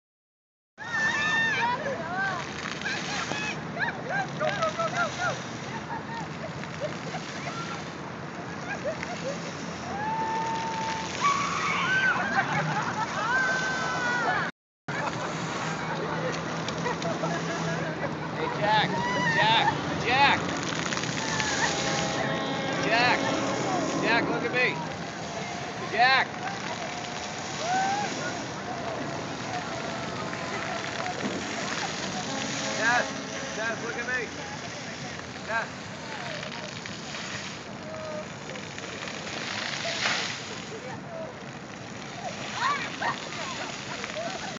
People getting wet in the fountain
People trying to Dodge the water in a fountain in Boston